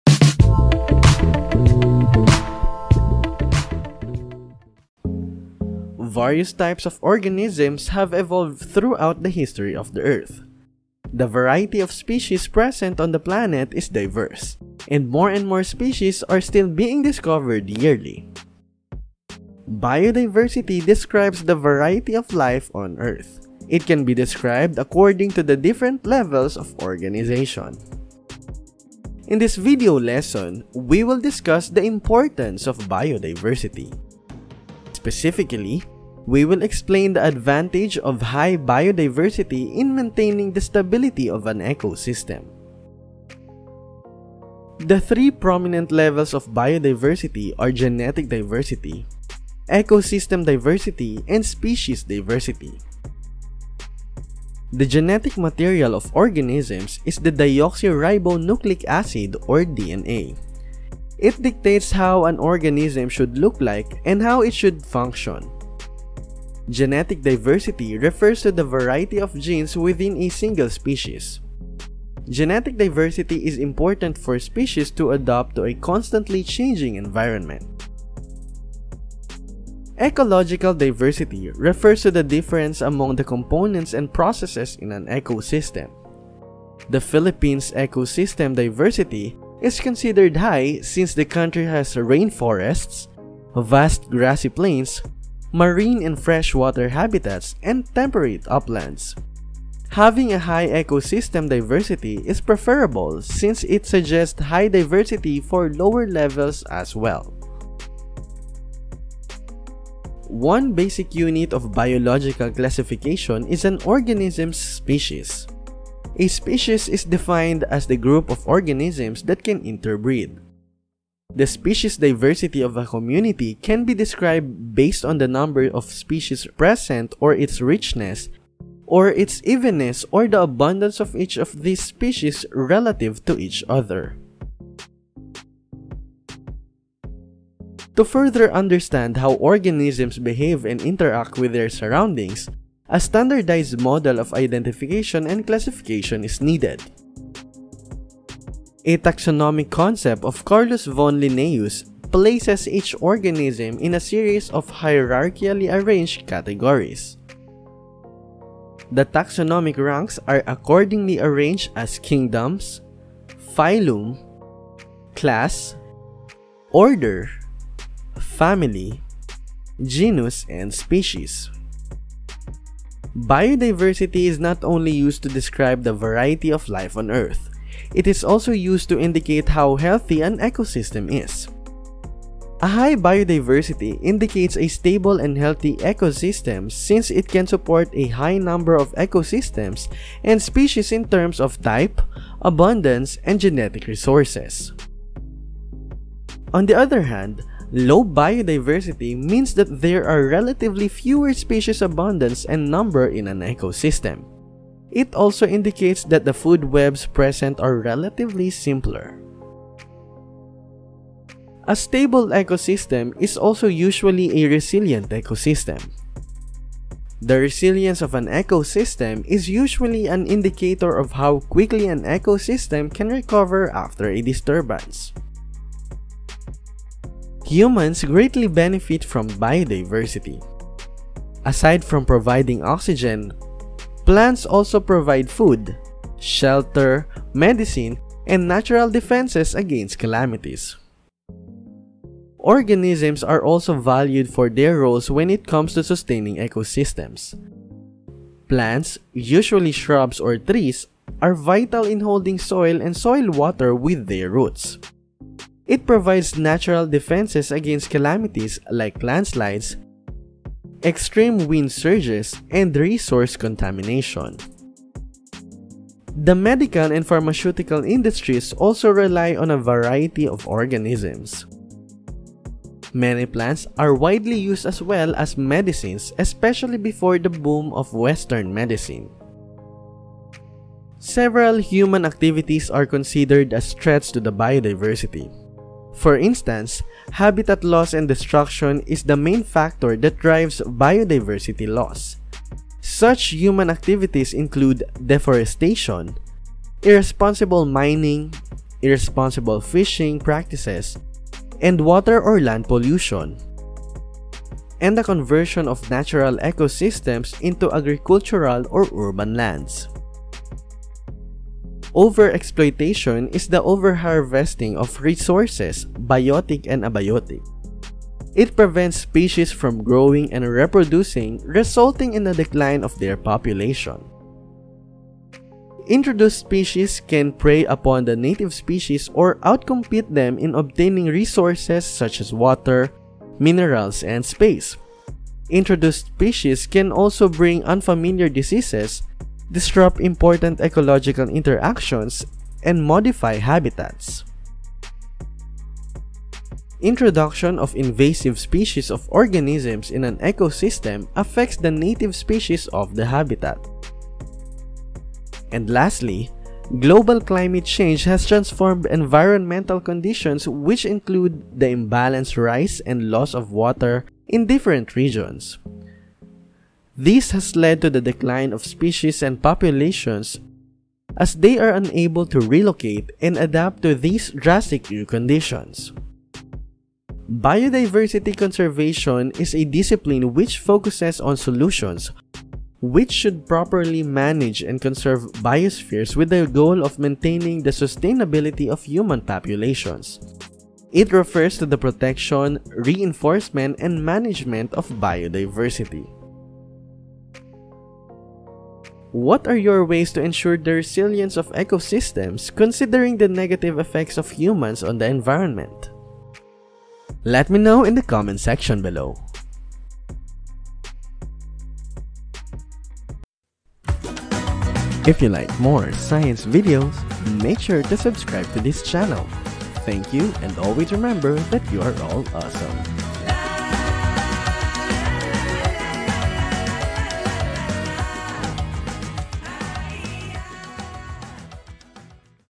BIODIVERSITY | AWESOME SCIENCE
This video explains the advantage of high biodiversity in maintaining the stability of an ecosystem.

Levels of Biodiversity and Importance of Biodiversity